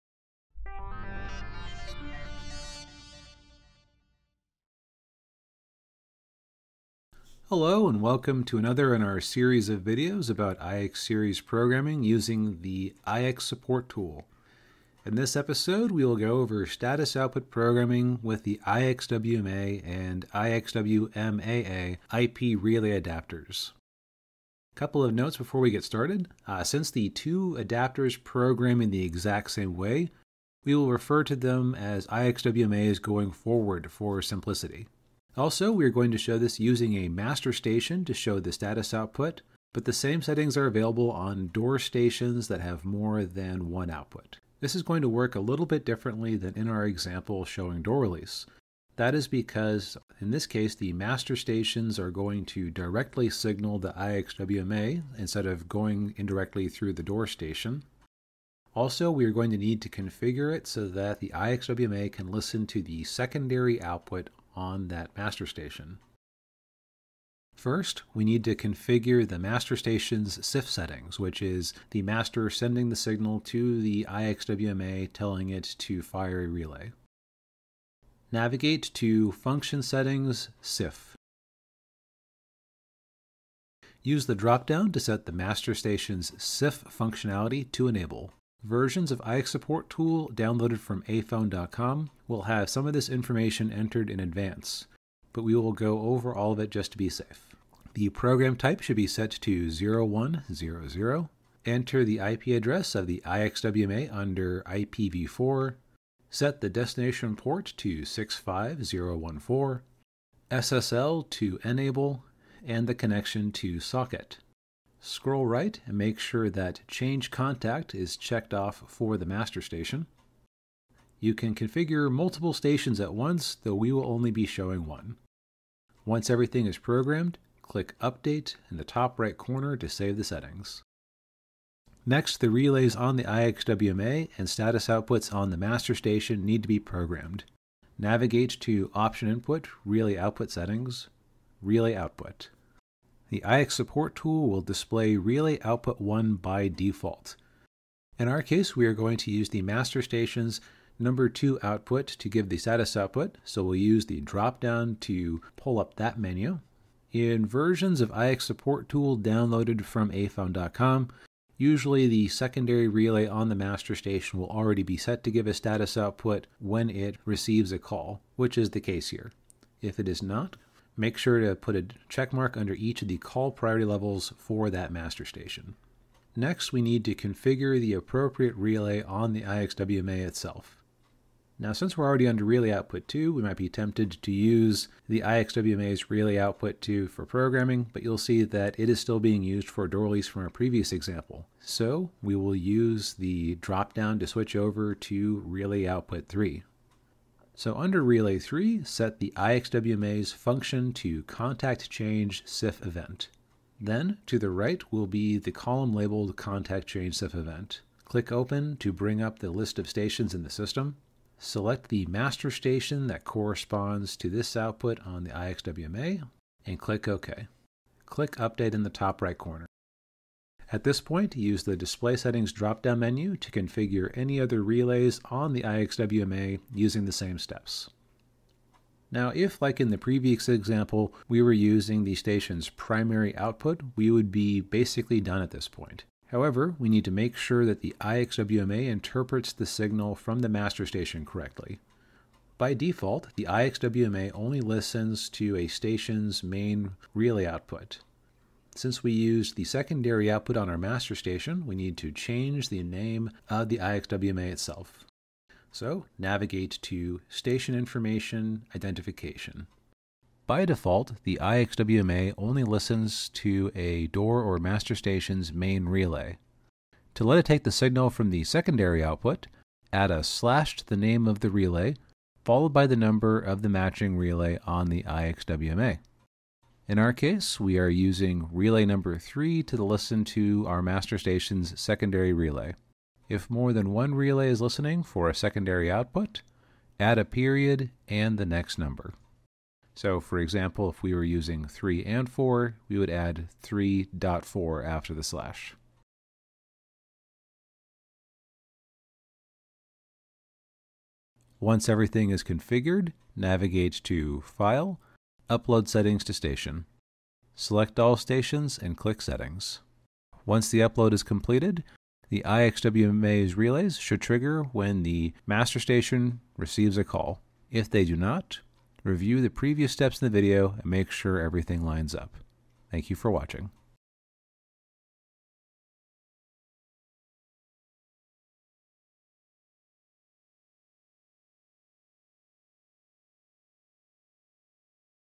IX Programming Episode 11: Status Outputs with the IXW-MA
Episode 11 of a series of videos about IX Series programming. This episode covers programming the IXW-MA IP relay adaptor for status outputs.